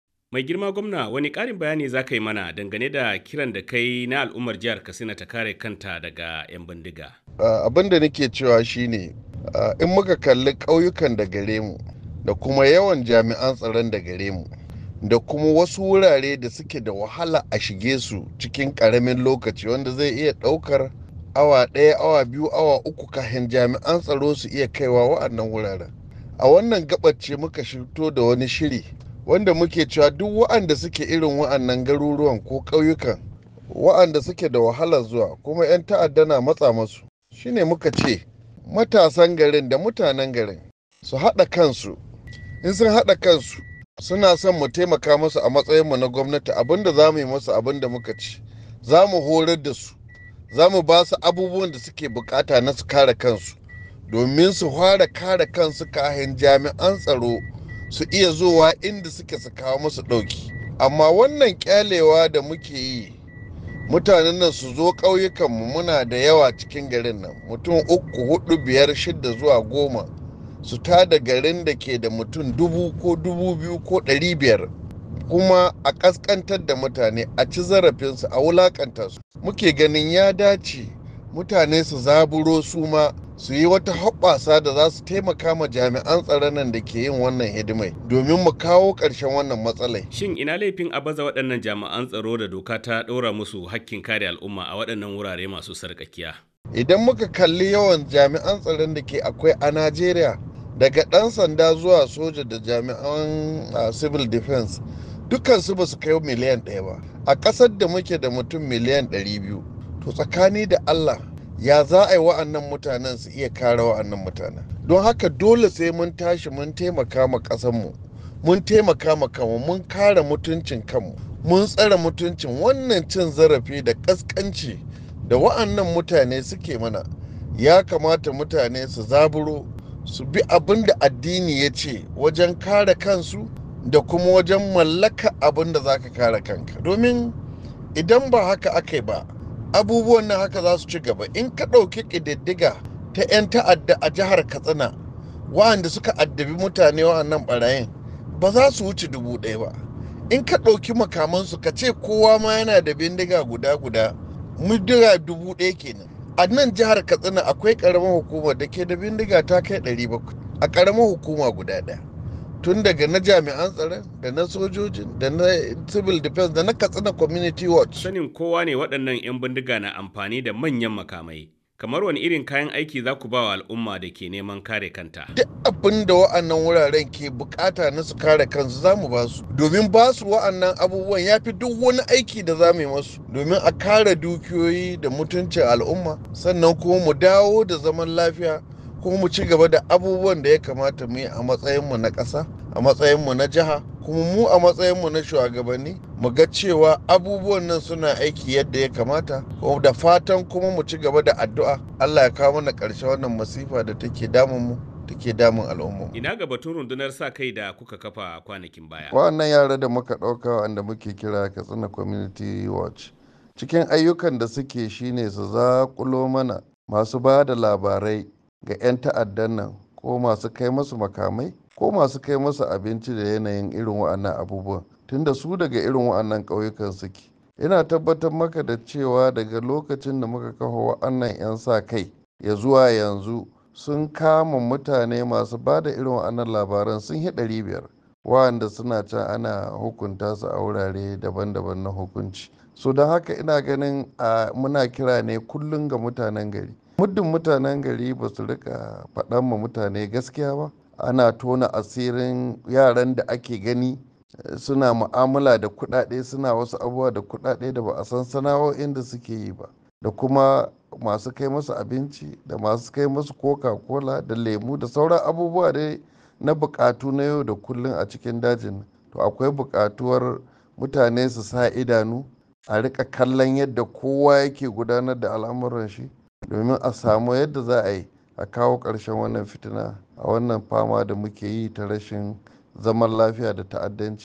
Gwamna Dikko Radda ya buƙaci al’umma da su jajirce wajen kare kansu daga ƴan bindiga… • RFI Hausa
Gwamnan jihar Katsina a Najeriya, Malam Dikko Umar Radda ya buƙaci al’umma da sa jajirce wajen kare kansu daga hare-haren ƴan bindiga da masu garkuwa da mutane, tare da bai wa jami’an tsaro haɗin kai don kawo  ƙarshen matsalolin tsaro da suka addabi yankin.

Gwamnan ya ce ƴan bindiga ba za su iya gudanar da ayyukansu cikin walwala ba, face tare da haɗin gwiwar wasu ɓata-gari a cikin al’umma. A zantawarsa da Ahmed Abba, gwamna Dikko Radda, ya yi ƙarin haske kan wannan kira.